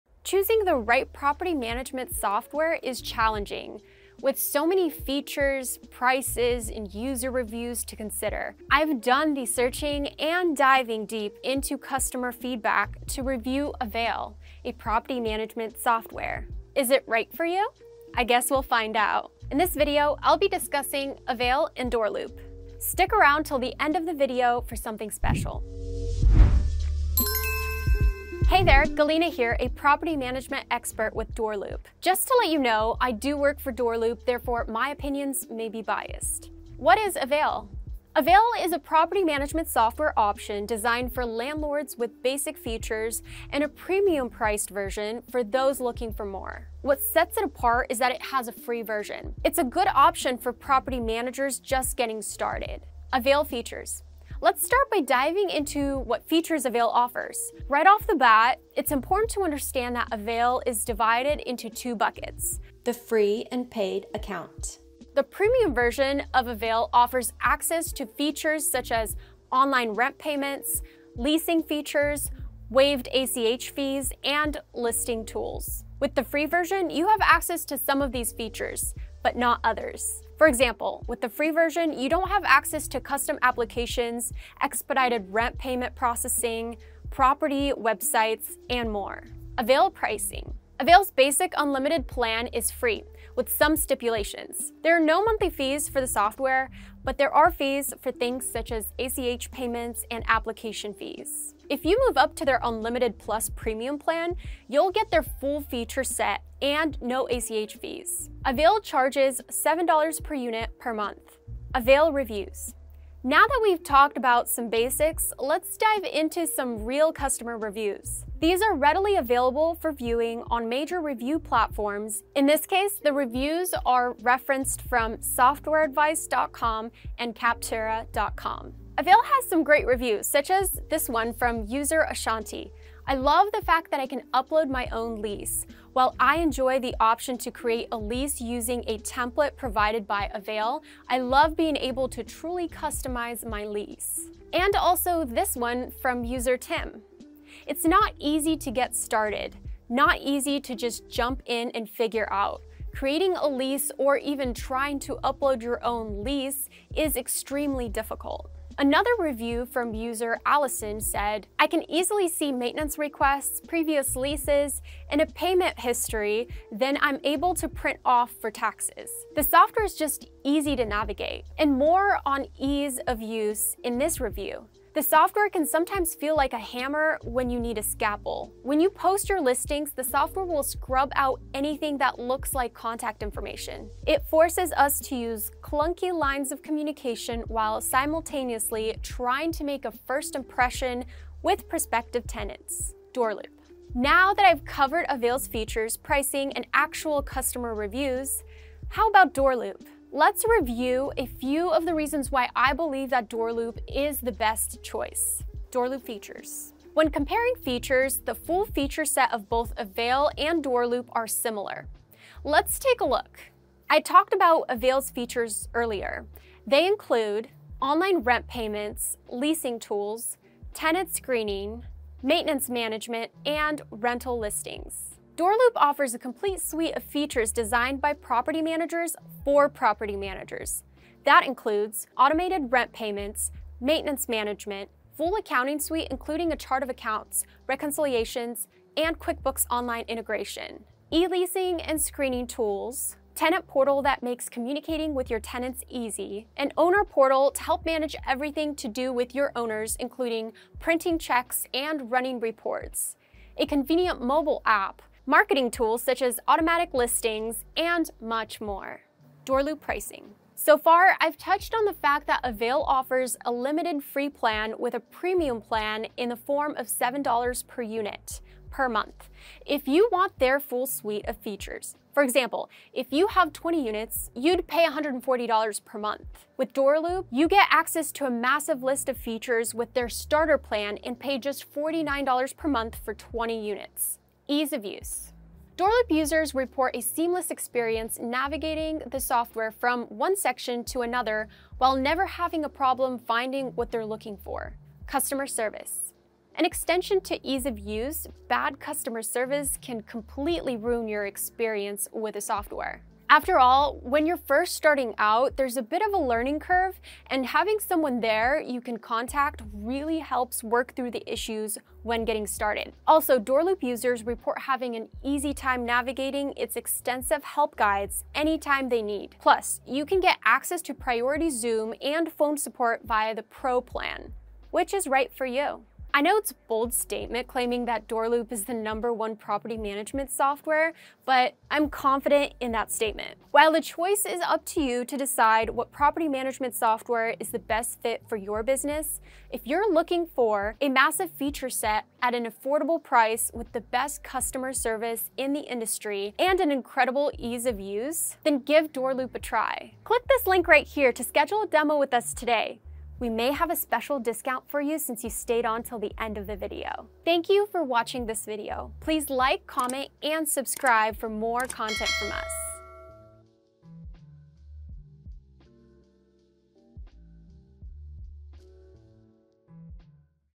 DoorLoop vs. Avail Reviews, Pricing, Features, & Alternatives
Choosing the right property management software is challenging, with so many features, prices, and user reviews to consider.  In this video, I will go over Avail and DoorLoop. Learn more about which property management software is right for you!

0:00 Overview
0:38 What is Avail
0:53 Avail Features
1:30 Avail Pricing
1:52 Avail Reviews
3:14 DoorLoop
3:26 DoorLoop Features
4:20 DoorLoop Pricing
04:45 Ease of Use
4:55 Customer Service
5:25 Which Is Right for You?
5:51 Schedule a Free Demo

Category: Software ‍‍